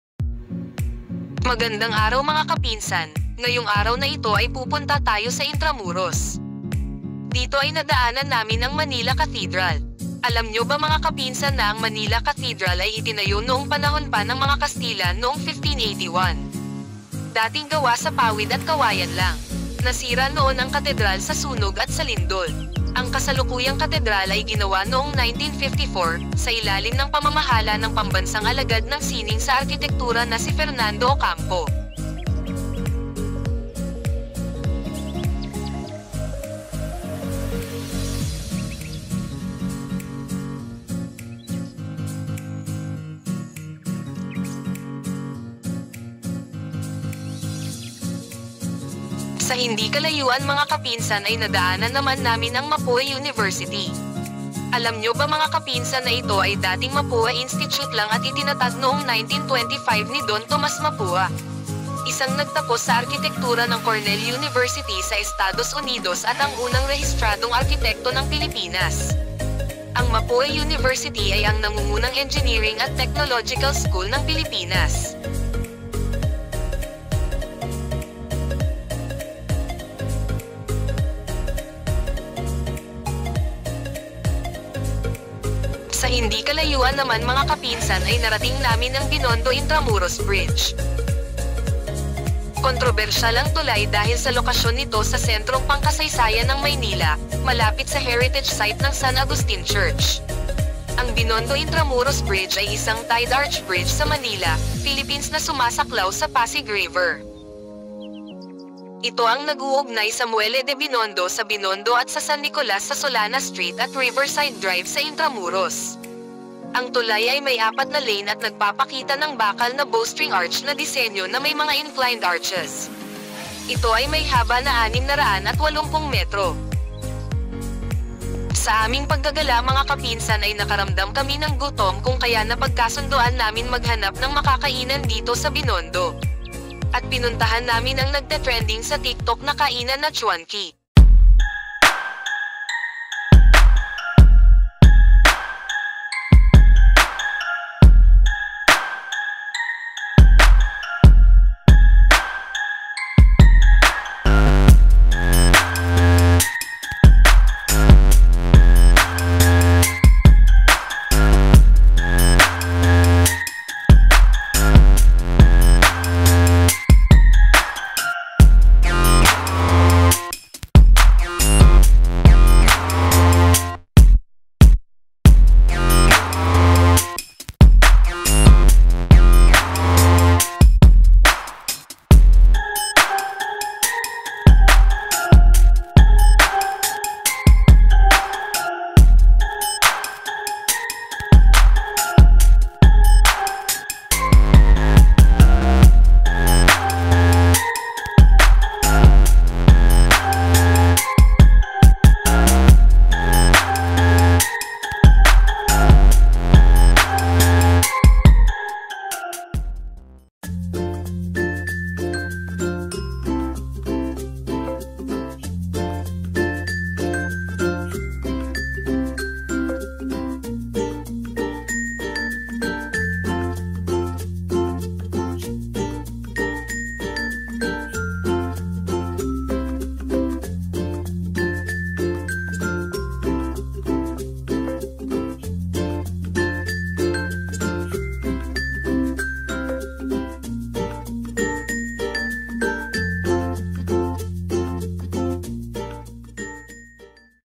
d Kasin's @ Intramuros /Binondo /ChuanKee - biglaang gala
In the Philippines, there’s no better place to chow down authentic Chinese food than Binondo. After all, it’s the oldest Chinatown in the world, dating back to the 16th century when Chinese merchants began to settle in the country.

Binondo remains the beating heart of the Chinese-Filipino community with plenty of iconic hotspots, like the Binondo Church and Plaza San Lorenzo Ruiz. For locals and travelers, it is also hands down the best destination in Manila for anyone itching for an amazing food trip.

Chuan Kee is one of the oldest fast food joints in Binondo, setting up the turo-turo or cafeteria style restaurant about seven decades ago and remaining a crowd favorite until today. Expect delicious Chinese comfort food with the best-sellers being kiampong (salty rice), maki soup, and pork asado.

The Binondo–Intramuros Bridge is a tied-archbridge in Manila, Philippines that spans the Pasig River. It connects Muelle de Binondo in Binondo and in San Nicolas to Solana Street and Riverside Drive in Intramuros.The bridge has four lanes and exhibits a steel bowstring arch design with inclined arches. It has a length of 680 meters (2,230 ft).

Mapúa University (Filipino: Pamantasang Mapúa), also known simply as Mapúa or MU, is a private research-oriented non-sectarian university locatedin MetroManila, Philippines. The university was founded in 1925 by the first registered Filipino architect, Tomás Mapúa, a graduate of Cornell University in New York. In 2000, the university was acquired by the Yuchengco Group of Companies.

Ang Manila Cathedral ay ginawa pa noong panahon ng mga kastila at Ang simbahan ng Manila Cathedral na kilala din bilang Manila Metropolitan Cathedral-Basilica (MMCB) ay isa sa mga paboritong pinagdadausan ng kasal dahil sa ganda ng labas at loob ng simbahan. Ang arkitektura ng harap ng simbahan ay Romanesque, isang pamana ng kasaysayan na lubos na umaakit sa mga turista. Ang napakagandang altar ay handog at pagkilala sa Our Lady of the Immaculate Conception, angkop sa sumpaan ng dalawang nagmamahalan. Ang Katedral ay gawa sa arikitekturang Romanesque-Byzantine at may pormang Baroque. Ang patsada ay binubuo ng dalawang palapag na bloke at may tatlong pasukan na may mga ukang arko. Ang mga pintuan ay yari sa tanso na inukit nina Alessandro Monteleone at Francisco Nagni. Nakadikit ang kampanaryo sa kanang panig ng gusali.
Itinayo ang Manila cathedral noong 1581, dating gawa sa pawid at kawayan, nasira noon ang katedral sa sunog at sa lindol ilang beses na din napagwa ang katedral at dumaan ang ilang taon sinubuakng patibayin ang katedral para sa pag hahanda sa susunod na panahon pero sa malakas na lindol nasira muli ang simbahan, maraming beses na din inayos ang katedral sa maraming kadahilan pero hindi pa din sumuko ang mga taong nagayos sa katedral ng Maynila.
Ang kasalukuyang Katedral ay ginawa noong 1954, sa ilalim ng pamamahala ng Pambansang Alagad ng Sining sa Arkitektura na si Fernando Ocampo. Pinasinayahan ang nasabing katedral noong 8 Disyembre 1958 ng Arsobispo ng Maynila na si Rufino Jiao Santos. Itinaas ang ranggo ng katedral sa minor basilica noong 1981 ni Papa Juan Pablo II.